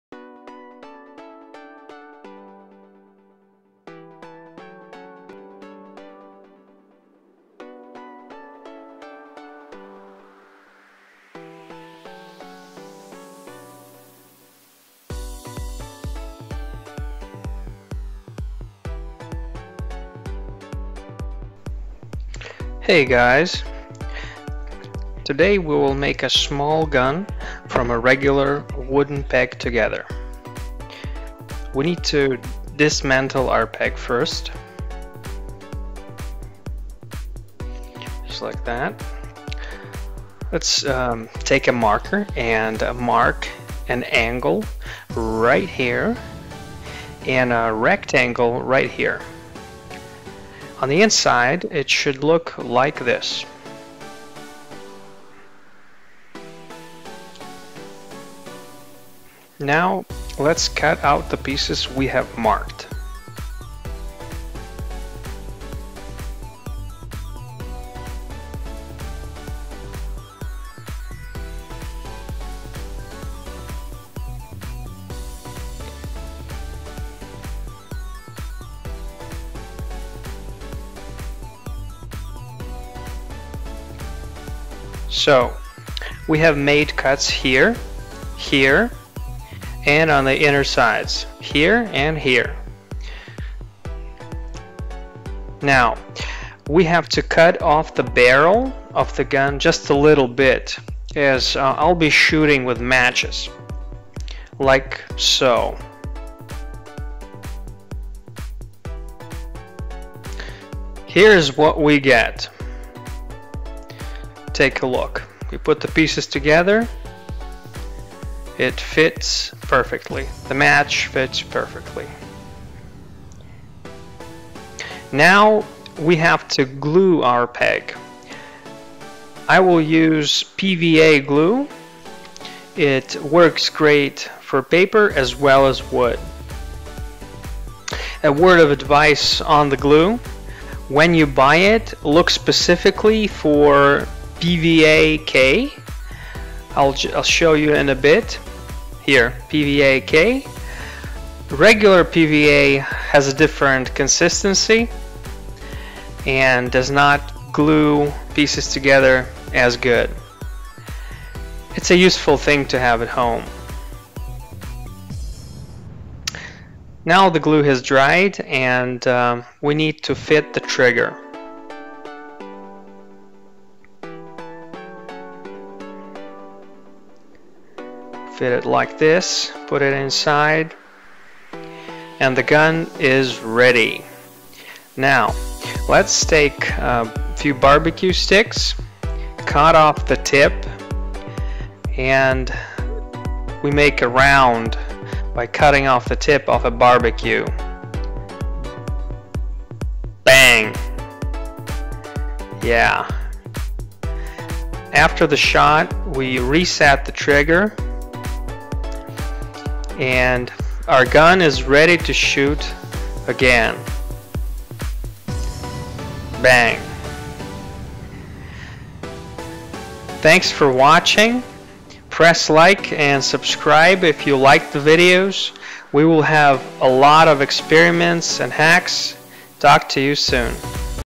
Mini Peg Gun DIY
Description: An interesting video on how to make a small gun out of a peg. It does pack some power and can pin trough an apple from 15 cm (5.9 inches).